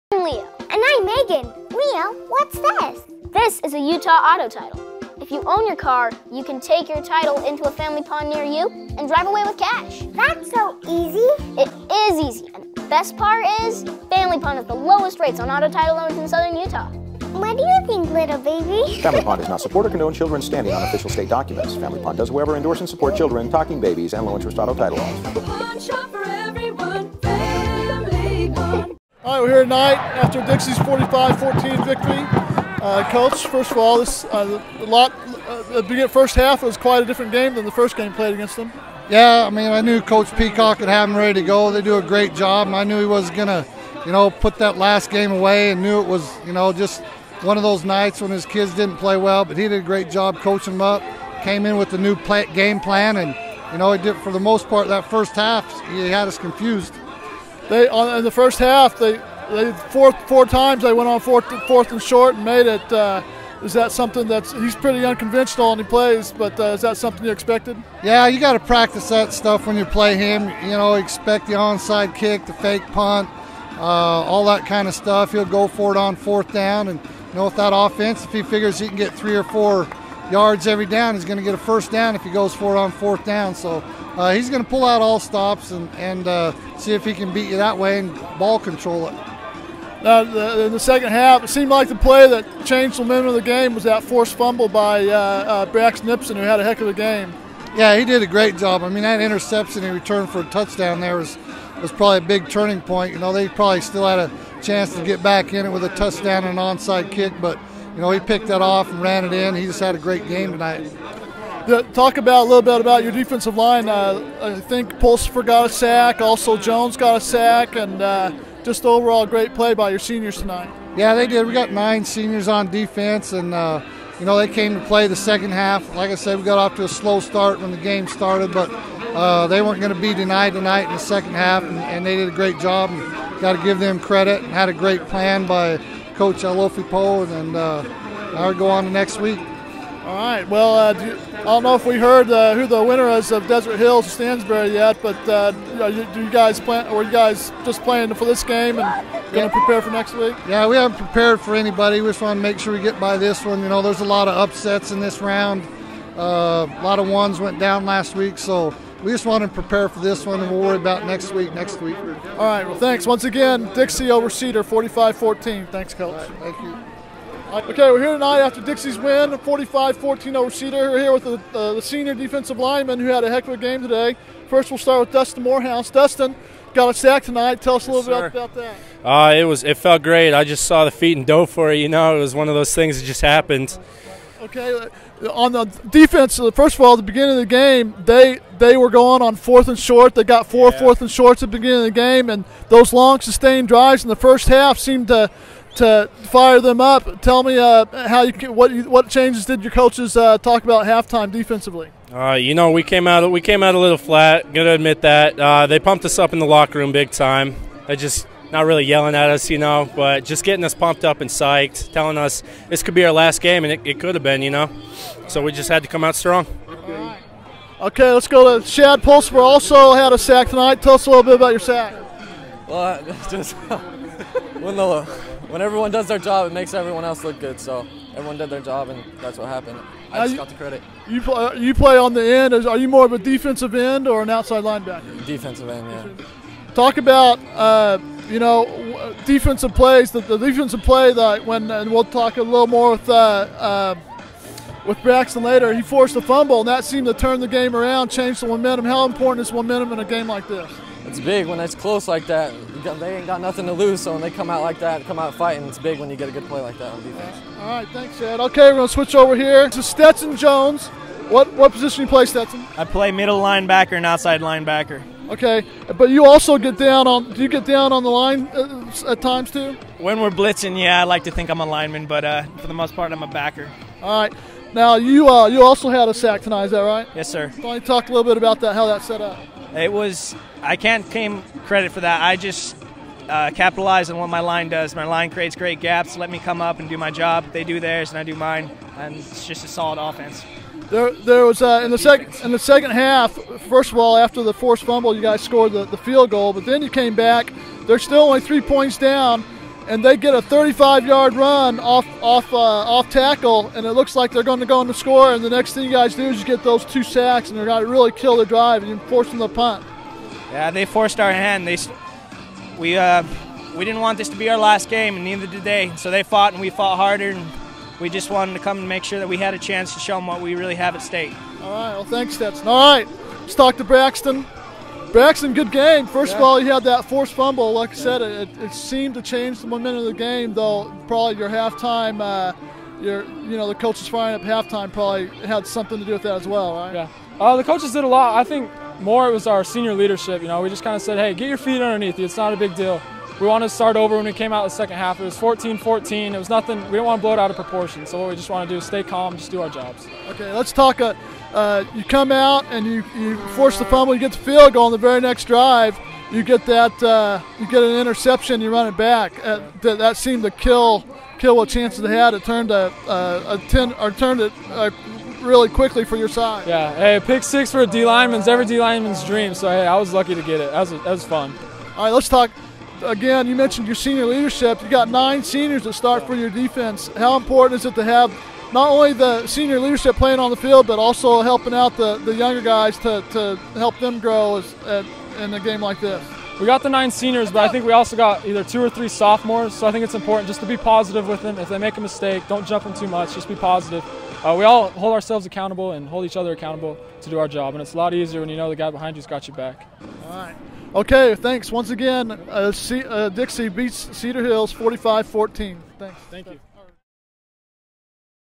Dixie Flyers vs Cedar Redmen 45 to 14
The Dixie High Flyers played the Ceder High Redmen 45 to 14 in their play-off game.